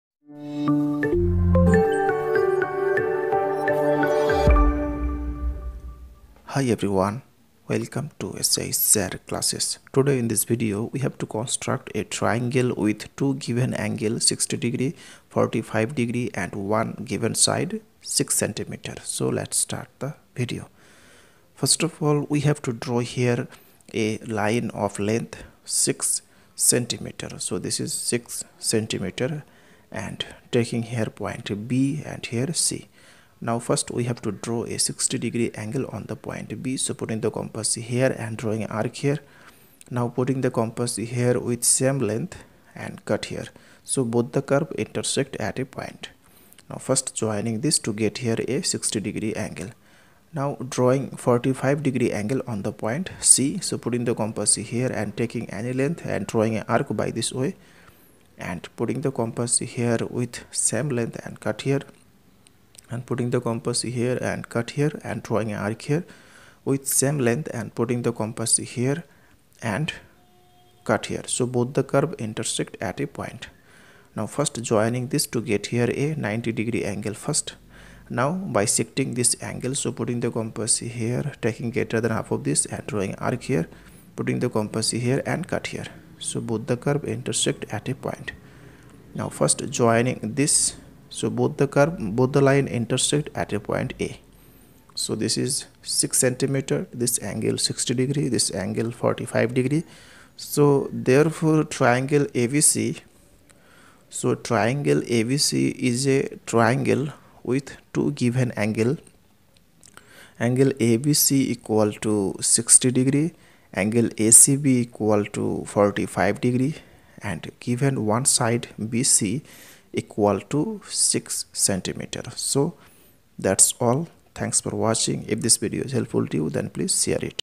How to construct a triangle with two angles 60° 45° and one side 6cm.shsirclasses.
In Easy Way how to construct a triangle with two angles 60° 45° and one side 6cm.shsirclasses. Geometrical Construction.